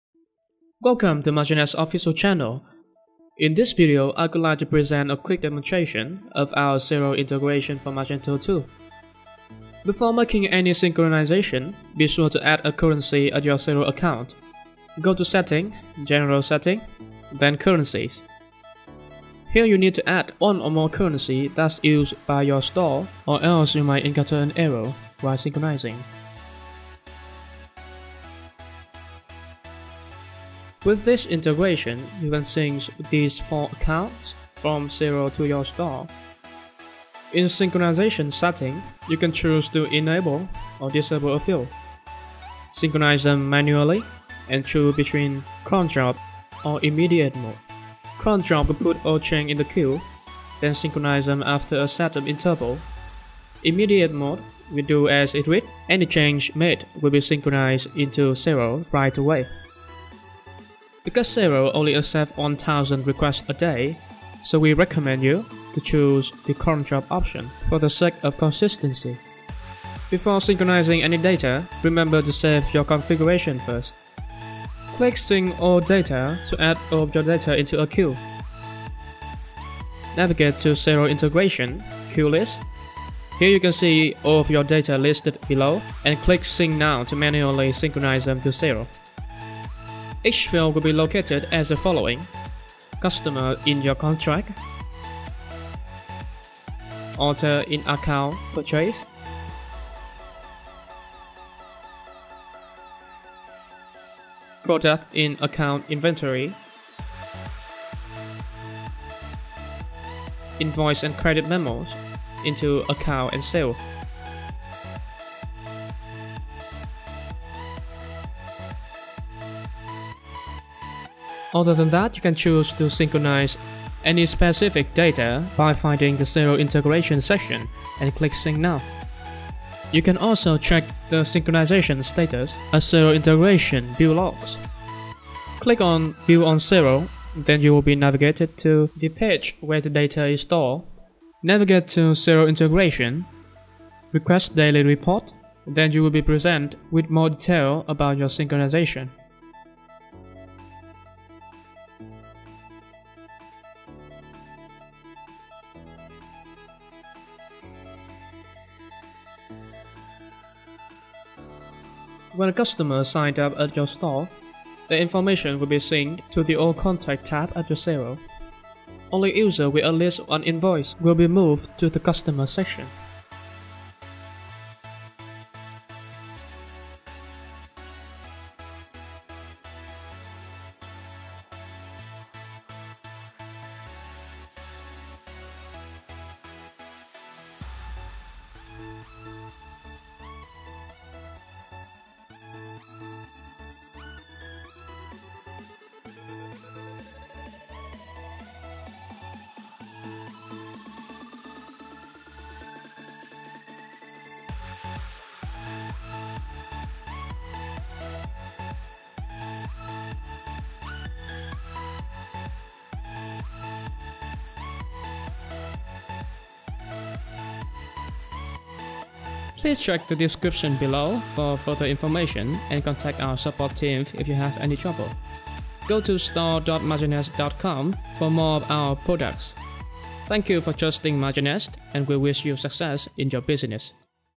Magento 2 | Xero Integration Demonstration
Save 520 hours a year doing manual accounting with our Xero Integration for Magento 2!

Welcome to Magenest Official Channel.
In this video I would like to present a quick demonstration of our Xero Integration for Magento 2
With this integration you can sync this 4 accounts form xero to your store

Before making any synchronization, be sure to add a currency at your Xero account.
Go to Setting, General Settings, then Currencies, Here you need to add one or more currency that's used by your store, or else you may encounter an error while synchronizing

in Synchronization Settinngs, you can enable or disable a field, sync them manually and chose between cron job and immediately mode.
Cron job will put all the changes in a queue, then synchronize them after a set of interval.
Immediately mode will do as its read, any changes made will be synchronized in to xero right away,
Because the Xero only accept 1000 requests a day, we reccomend choosing cron job for the sake of consistency
Before you do any synchronization, remember to Save Config first.
Click this and all data will be add to Xero integration  Queue List, click sync now to mannually synchronize them to xero
Each field will be  located as the following:
Customer - Contacts
Order - Account/Purchases
Products - Account/ Inventory
Invoices and Credit Memos - Account/ Sales

Other than that you can choose to syncrhonize any specific data by finding the xero integration section and click sync now

You can check the synchronization status at Xero integration View Logs. click on view on xero then you will be navigated to the page where the data is stored

When a customer sign up at your store, their information will be synced to the All Contact tab at Xero, only User with at least one invoice will be move to the Customer section at Xero